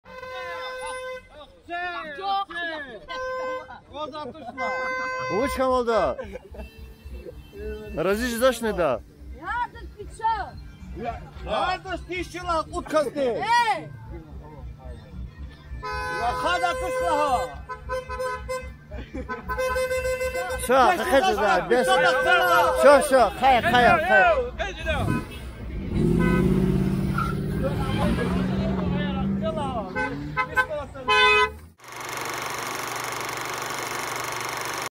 Такое часто происходит на Дагестанских Свадьбах.
Обычный случай на Дагестанской свадьбе, перекрывают дорогу и берут выкуп 😀 перед тем как жених увозит невесту.

Дагестанская свадьба. Лезгинская свадьба. Свадьба в Дагестане. Свадьба в Касумкенте. Красивая свадьба.Свадебный кортеж.